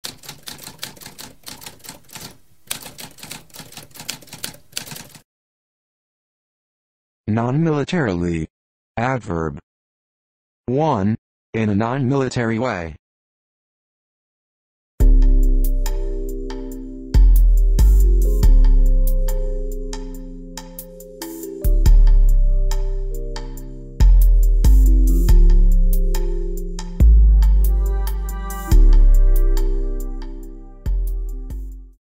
What does nonmilitarily mean?
A spoken definition of nonmilitarily.

Intro Sound:
Typewriter - Tamskp